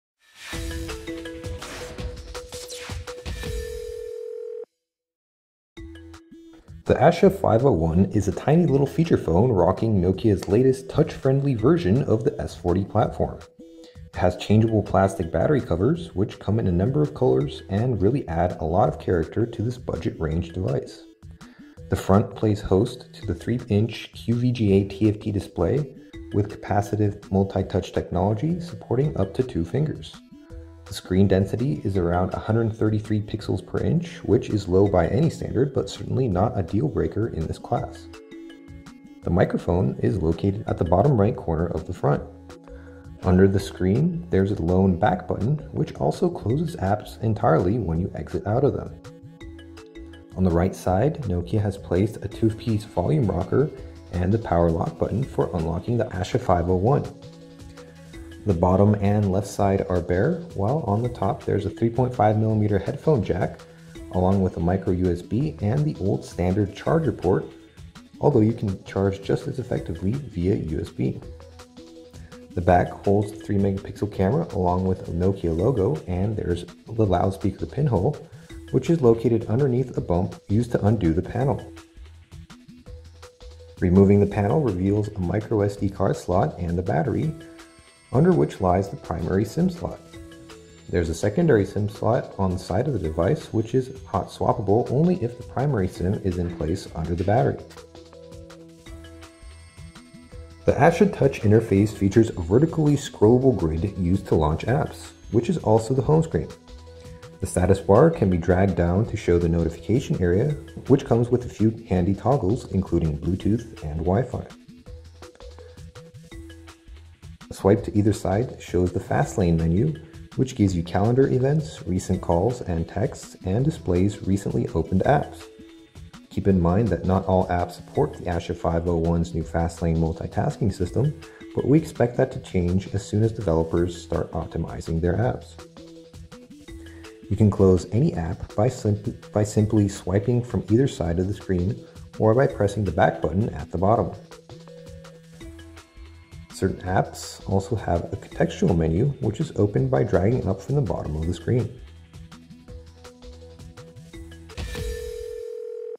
Nokia Asha 501 hands-on
This is a brief hands-on video showing off the S40 interface of the Nokie Asha 501. For more on the Asha 501, check out